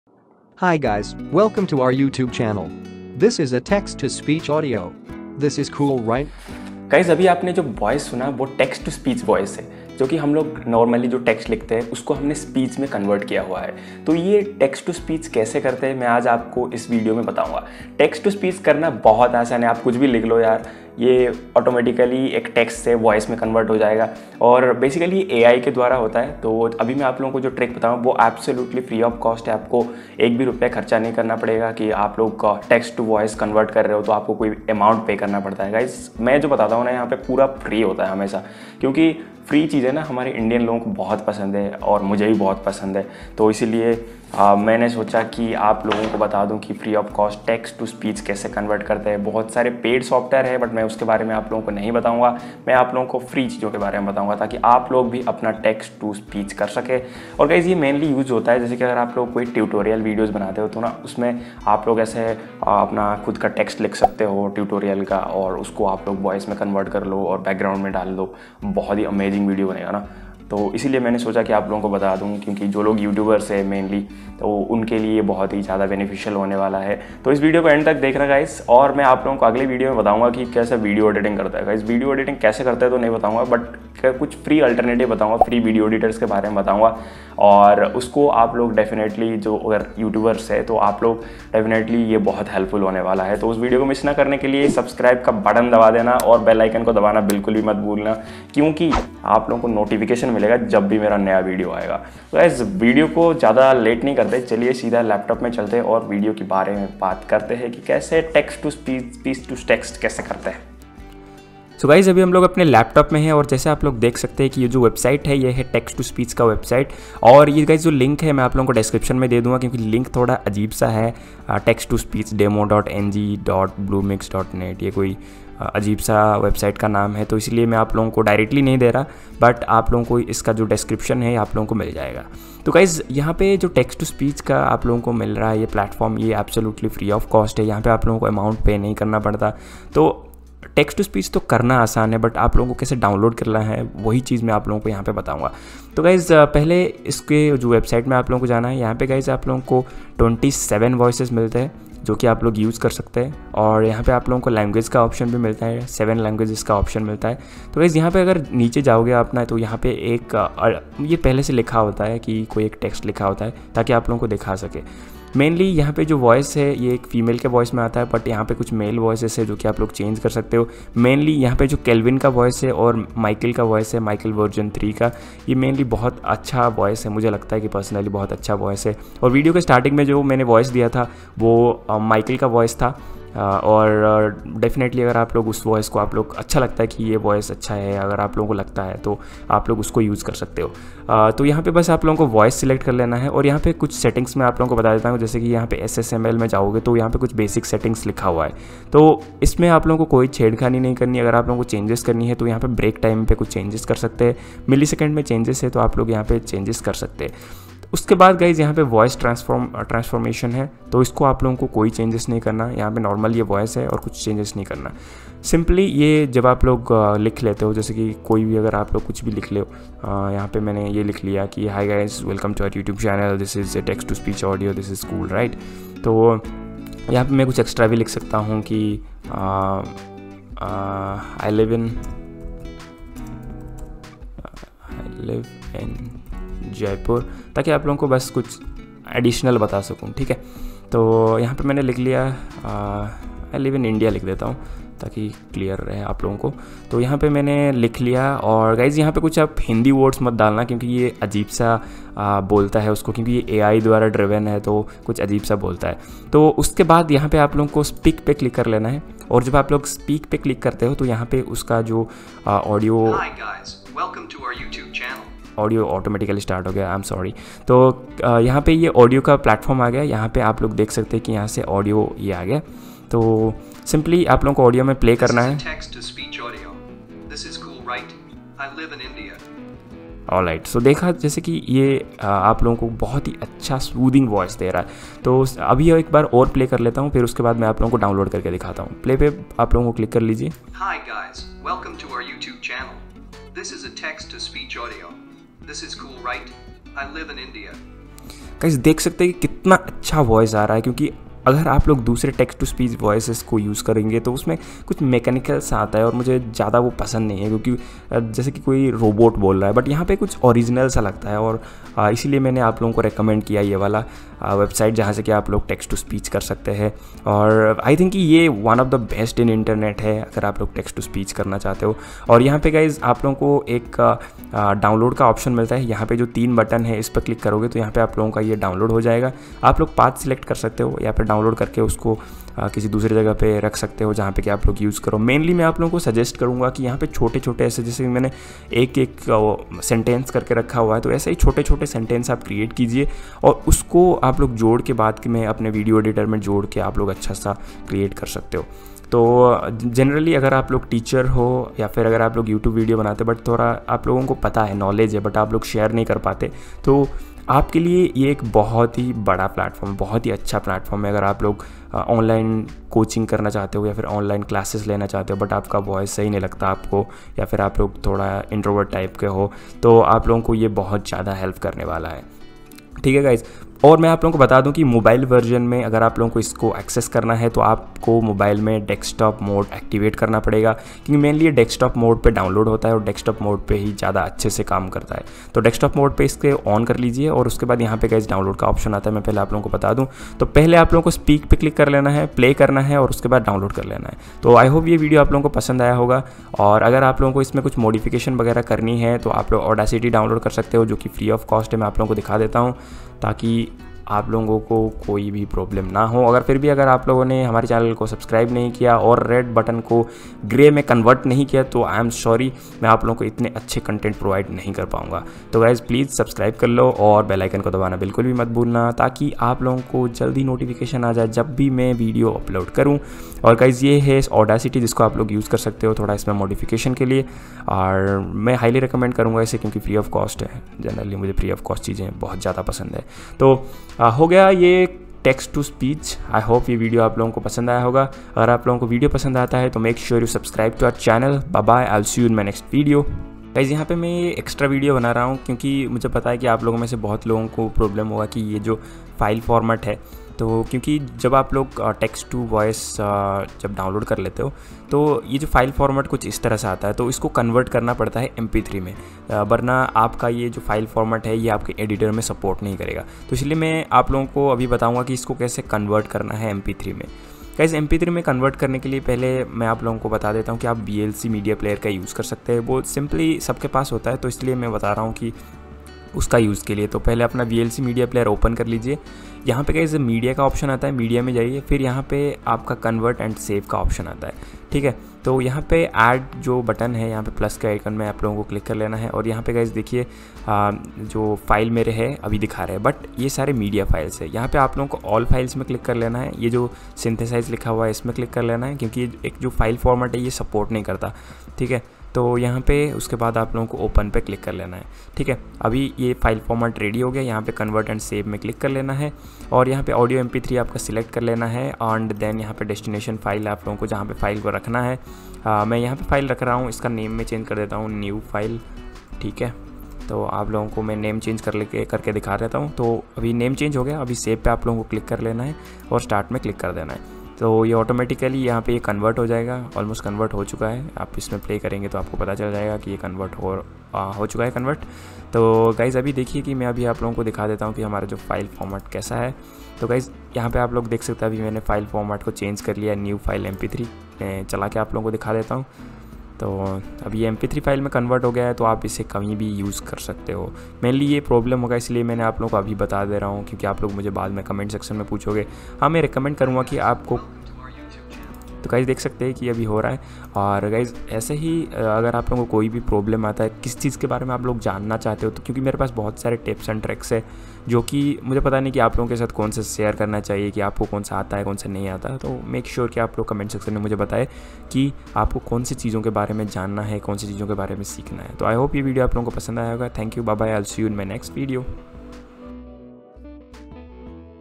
Text to speech Converter for YouTube Videos[Real Voice 2020]
In this video, I have talked about the text to speech converter for your Youtube Videos. Here I have explained and shown how to convert your text to a speech and use it in your youtube videos and tutorials. After watching this video you will also be able to produce good content like tutorials and educational content. This is my personal experience on youtube and I have shown how I use this tool for making videos.